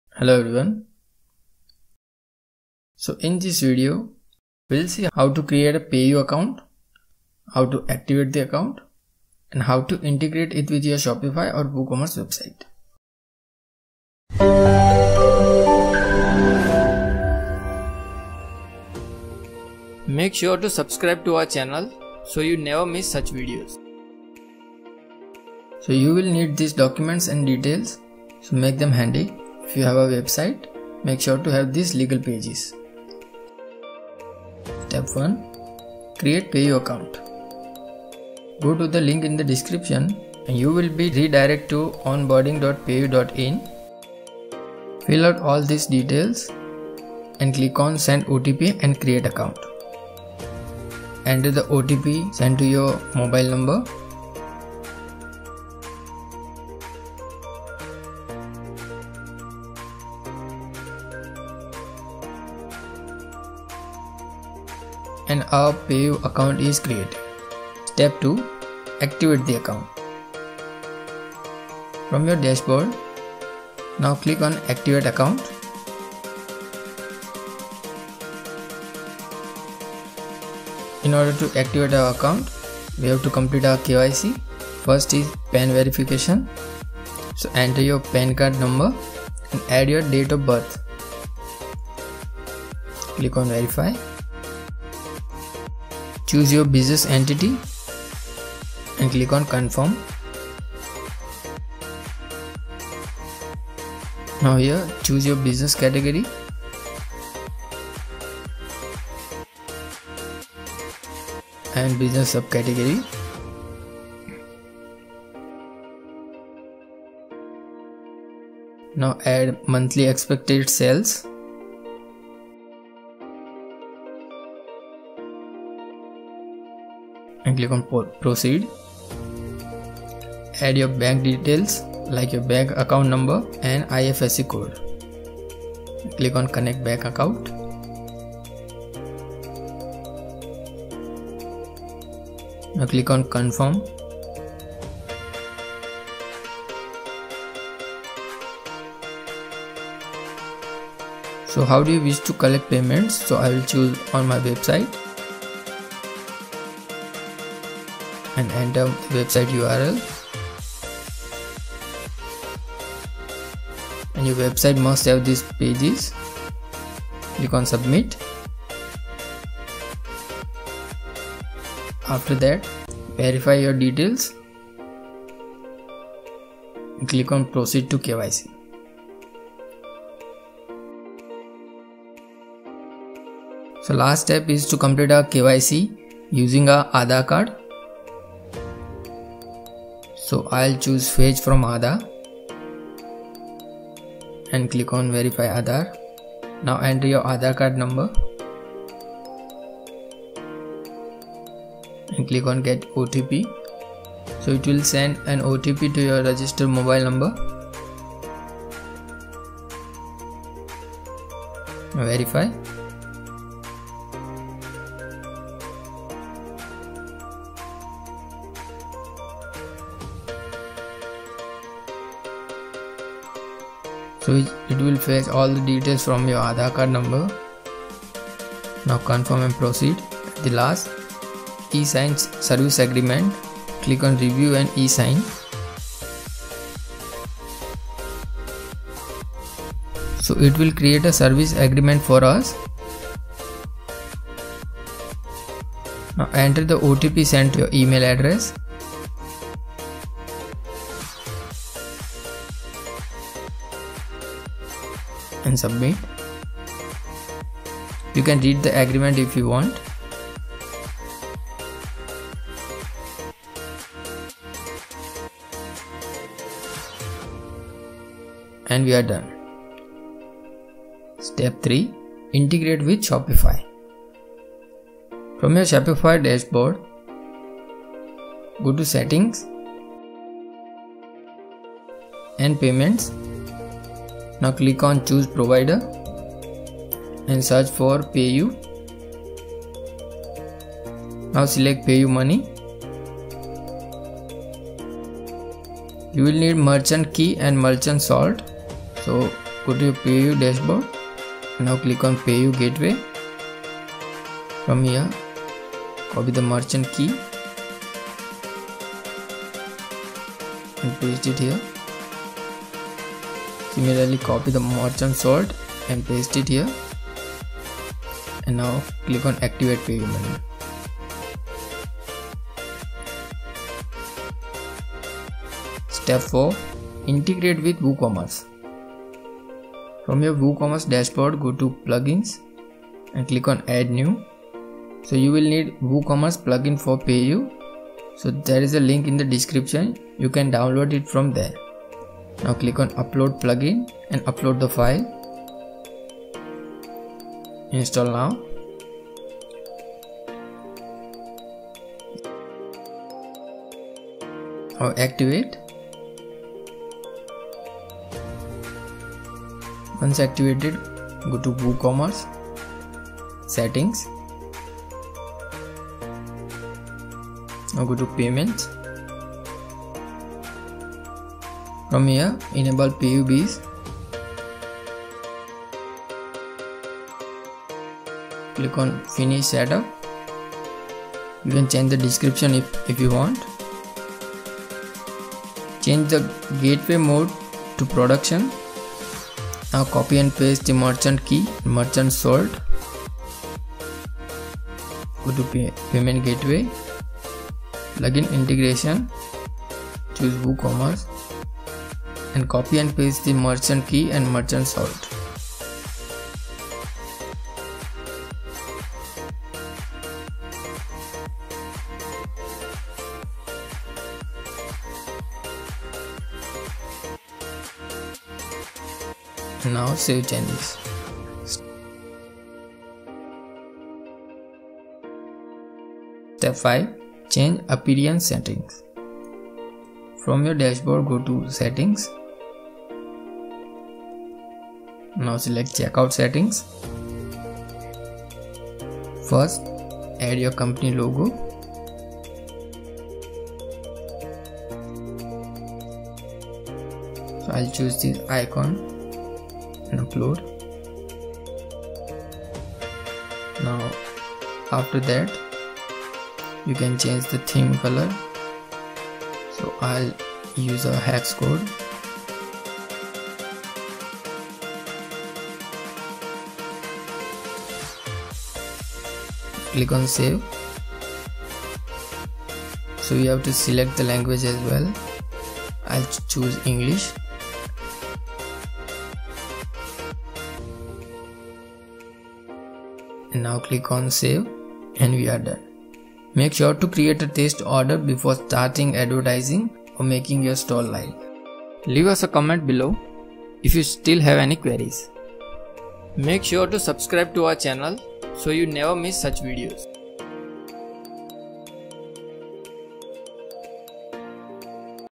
Create PayU Money Payment Gateway Account 2024 [ Step-by-Step ]
❤️ Hello Everyone
In this video, we'll see, How to create a PayU payment gateway account, how to activate the account, and how to integrate it with your Shopify or Woocommerce Website.
🎬 𝗧𝗮𝗯𝗹𝗲 𝗼𝗳 𝗖𝗼𝗻𝘁𝗲𝗻𝘁𝘀
0:00 - Intro
0:21 - Requirements
0:46 - Create an Account
2:35 - Account Activation
4:05 - Shopify Integration
5:22 - Woocommerce Integration
7:13 - Change Appearance Settings
7:35 - Important Note
📌 Follow these steps
👉 Step 1  ➜ Create a new PayU Account.

👉 Step 2  ➜ Fill out all the information and Complete the KYC to Activate the Account.

👉 Step 3  ➜ Go to Your Shopify Back-end (your domain/admin) ➜ Settings ➜ Payments ➜ Choose Payment Provider ➜ Choose PayU.
- Copy and Paste the Keys and Save.

👉 Step 4  ➜ Go to your WordPress website's backend dashboard to install PayU Plugin
To Access your WordPress website backend dashboard, go to "your domain/wp-admin".
- Install and Activate the Plugin(Razorpay)
- Woocommerce  ➜ Settings ➜  Payments
- Enable Razorpay and Click Manage

👉 Step 5 ➜ Change Appearance Settings